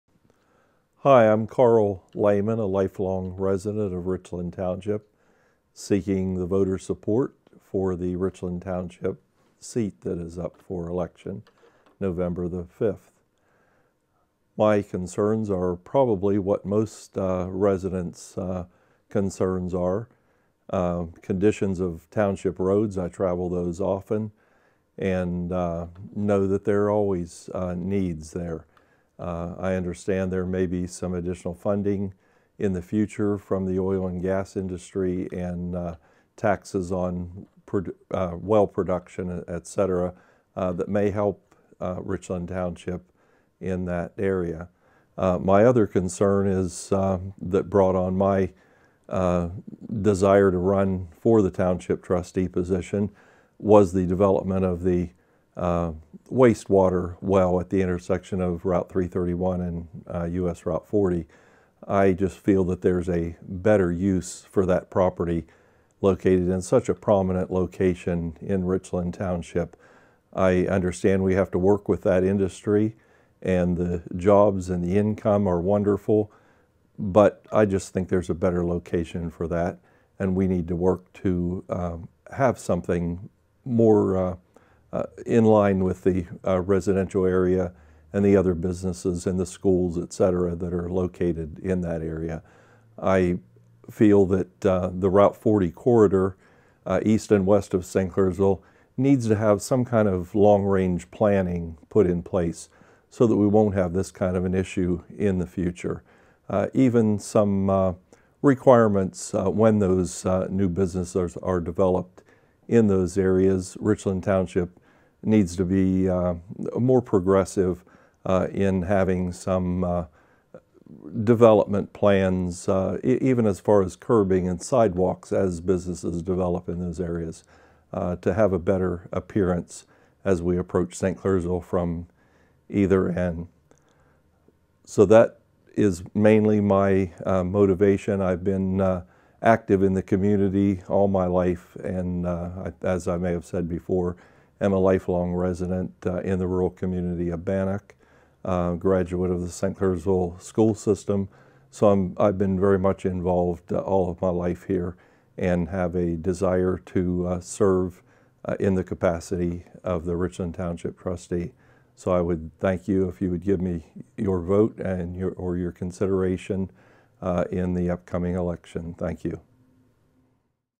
Carl Lehman candidate for Richland Township Trustee
Carl Lehman candidate for Richland Township Trustee.  This video does not constitute an endorsement of the candidate or his statements by ESV Teleproductions or Today's History: Upper Ohio Valley.